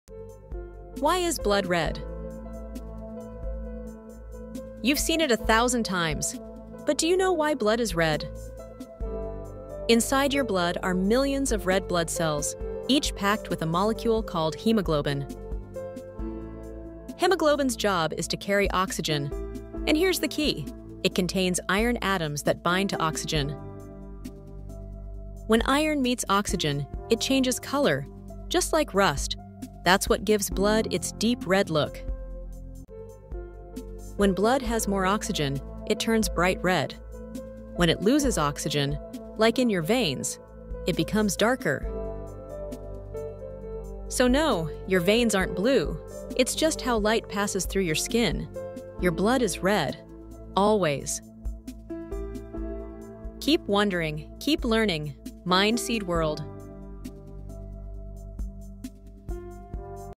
Why Is Blood Red? 🩸
Why is blood red? The answer lies in iron and oxygen.
Hemoglobin in your red blood cells binds with oxygen — and that chemical reaction gives blood its color.
It’s nature’s way of showing life in motion.

Keep wondering, keep learning. MindSeedWorld.